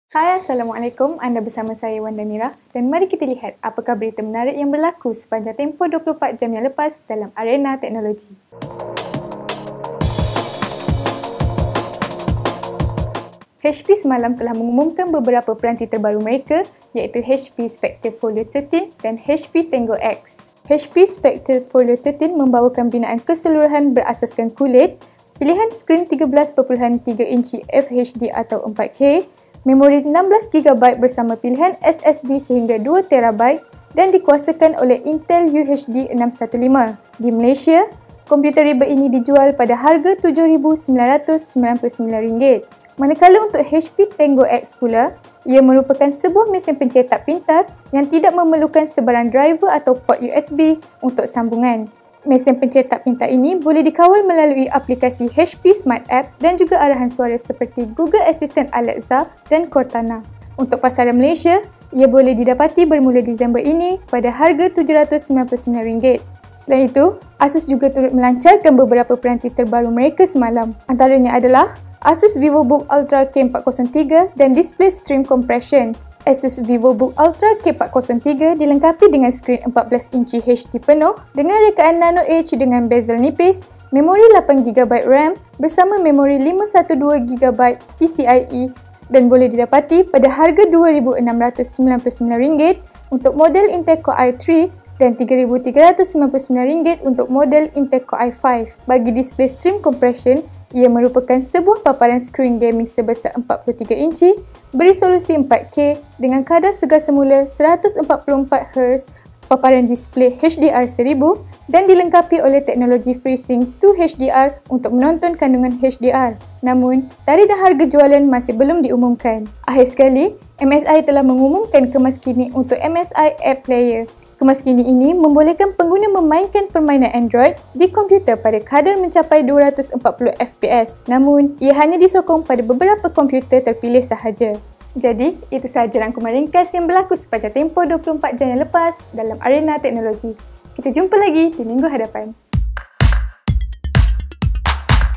Amanz24 - HP Spectre Folio 13, Asus VivoBook, MSI App Player
HP mengumumkan beberapa peranti terbaru mereka, iaitu HP Spectre Folio 13 dan HP Tango X untuk pasaran Malaysia. HP Spectre Folio 13 membawakan binaan keseluruhan berasaskan kulit, pilihan skrin 13.3 inci FHD atau 4K, memori 16GB bersama pilihan SSD sehingga 2TB dan dikuasakan oleh Intel UHD 615. Di Malaysia, komputer riba ini dijual pada harga RM7999.

Manakala untuk HP Tango X pula, ia merupakan sebuah mesin pencetak pintar yang tidak memerlukan sebarang driver atau port USB untuk sambungan. Mesin pencetak pintar ini boleh dikawal melalaui aplikasi HP Smart App dan juga arahan suara seperti Google Assistant Alexa dan Cortana. Untuk pasaran Malaysia, ia boleh didapati bermula Disember ini pada harga RM799.

Selain itu, Asus juga turut melancarkan beberapa peranti terbaru mereka semalam. Antaranya adalah Asus VivoBook Ultra K403 dan Display Stream Compression. Asus VivoBook Ultra K403 dilengkapi dengan skrin 14 inci HD penuh dengan rekaan NanoEdge dengan bezel nipis, memori 8GB RAM bersama memori 512GB PCIe dan boleh didapati pada harga RM2699 untuk model Intel Core i3 dan RM3399 untuk model Intel Core i5.

Bagi Display Stream Compression, ia merupakan sebuah paparan skrin gaming sebesar 43 inci, beresolusi 4K dengan kadar segar semula 144Hz, paparan Display HDR1000 dan dilengkapi oleh teknologi FreeSync 2 HDR untuk menonton kandungan HDR. Namun tarikh dan harga jualan masih belum diumumkan.

Akhir sekali, MSI telah mengumumkan kemaskini untuk MSI App Player. Kemaskini ini membolehkan pengguna memainkan permainan Android di komputer pada kadar mencapai 240FPS. Namun, ia hanya disokong pada beberapa komputer terpilih sahaja.